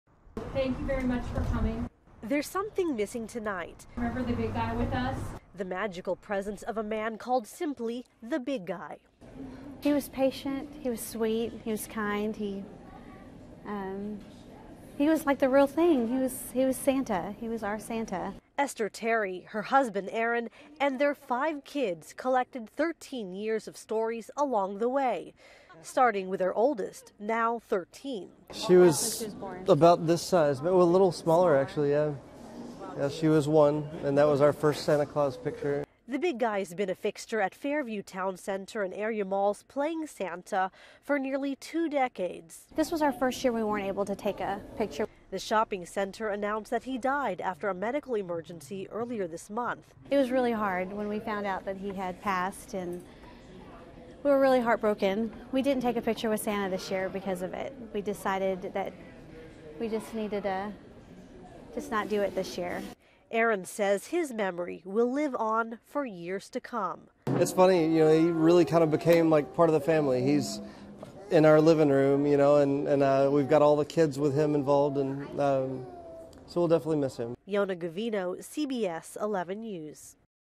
Dozens say farewell to mall Santa who died before Christmas
Dozens gathered Tuesday evening to say farewell to a man they knew simply as The Big Guy, who has been a fixture at Fairview Town Center and area malls playing Santa for nearly two decades, CBS DFW reports.